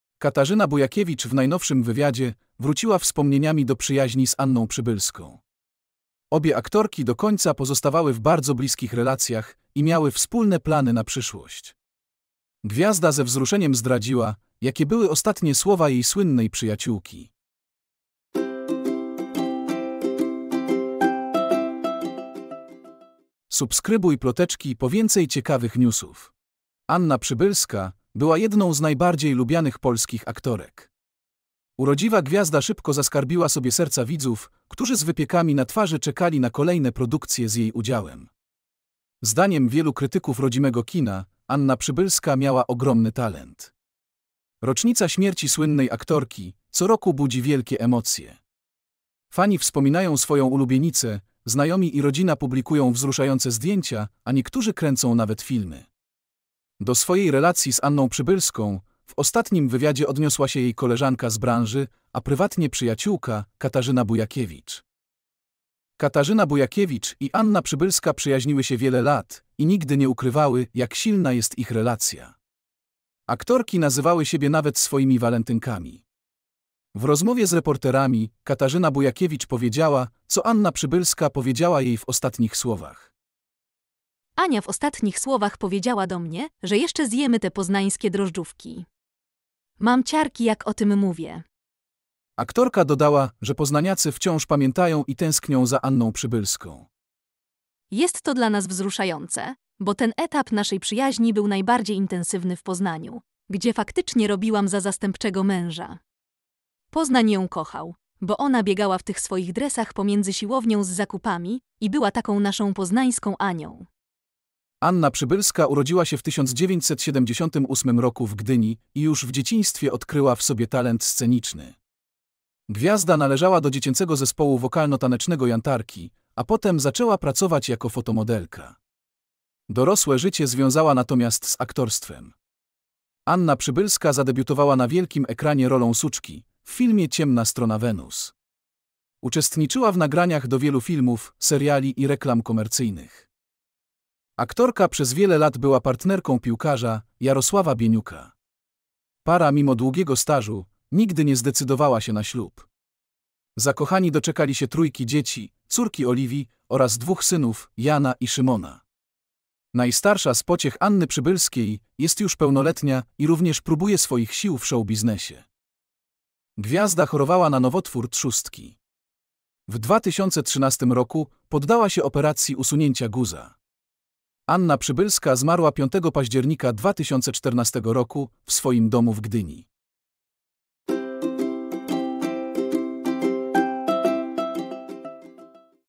Ostatnie słowa Anny Przybylskiej. Katarzyna Bujakiewicz wspomina: "Mam ciarki!"
W rozmowie z reporterami, Katarzyna Bujakiewicz powiedziała, co Anna Przybylska powiedziała jej w ostatnich słowach.